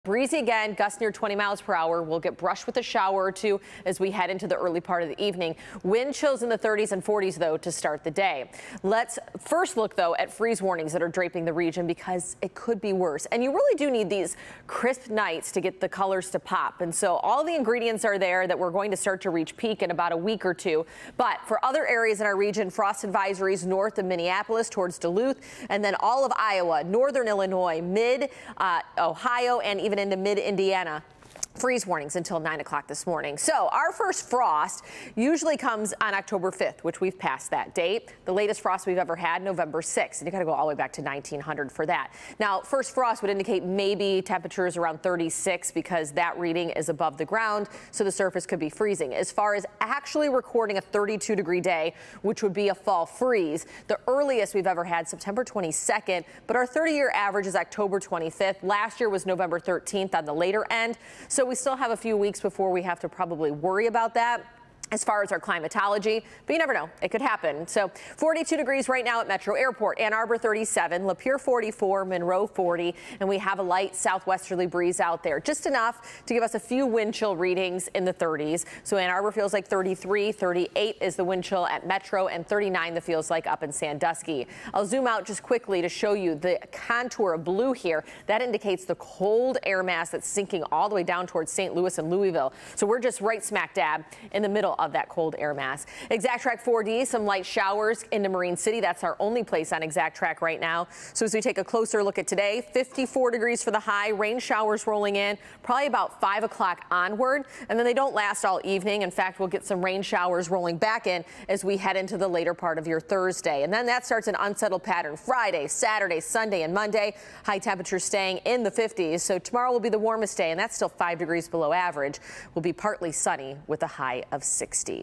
Rain showers possible amid cool week in Metro Detroit: What to expect and when
We're tracking continuing cool temps in Metro Detroit this week as rain chances ramp up Tuesday. More rain is possible Thursday through the weekend.